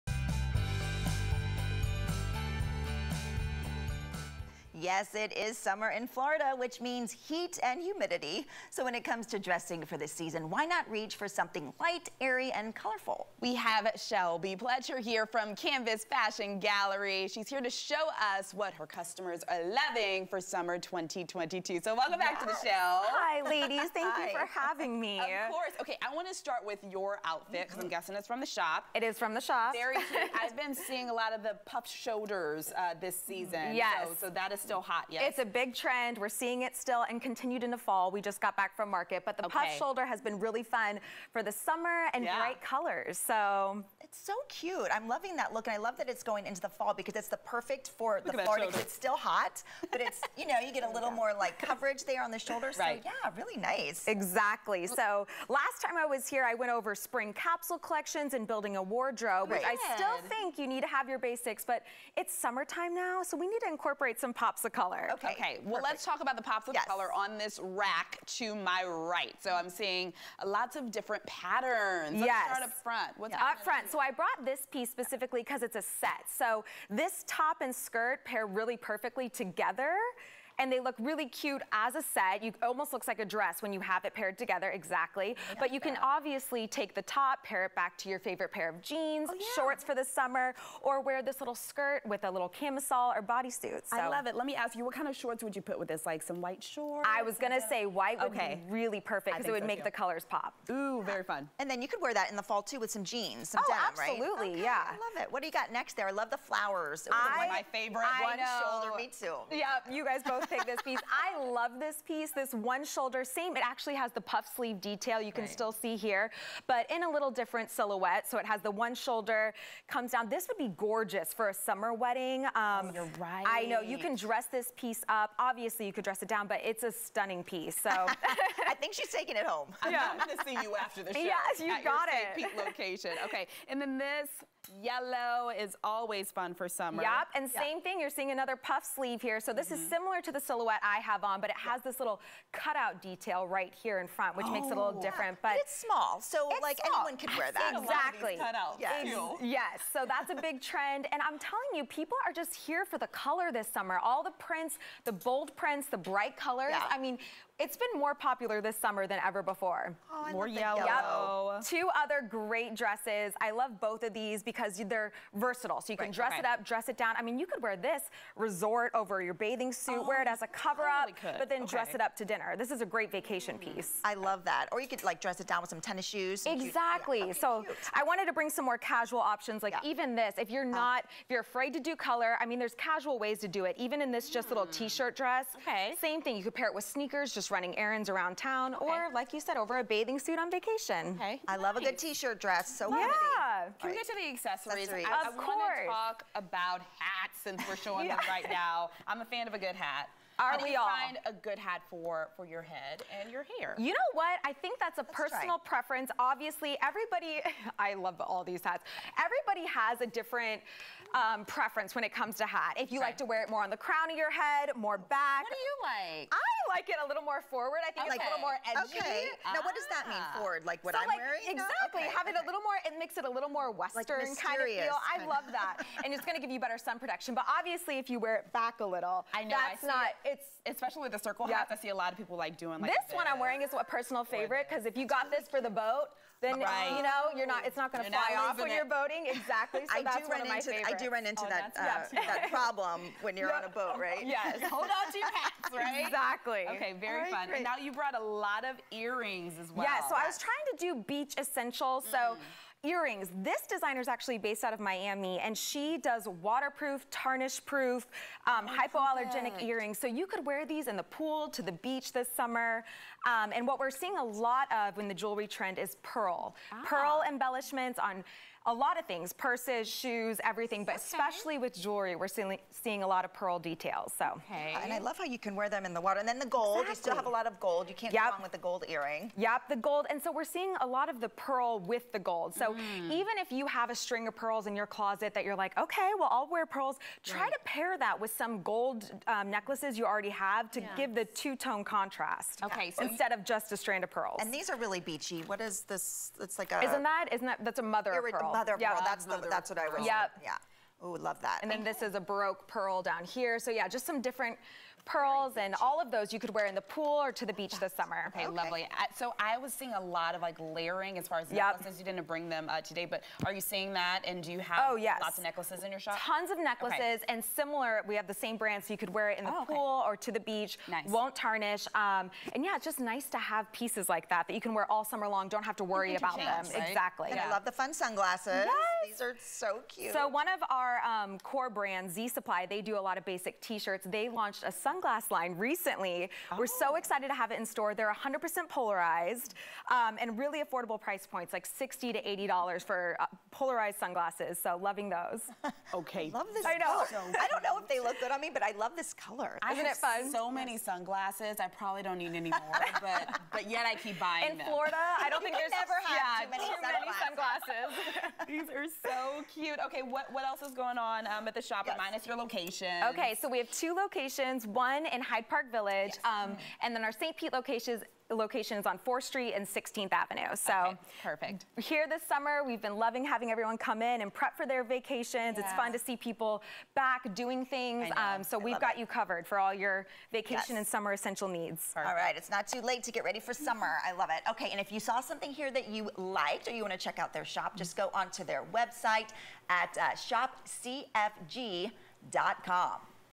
Dressing for Summer 2022
Shelby Pletcher from Canvas Fashion Gallery shows us what customers are loving for Summer 2022.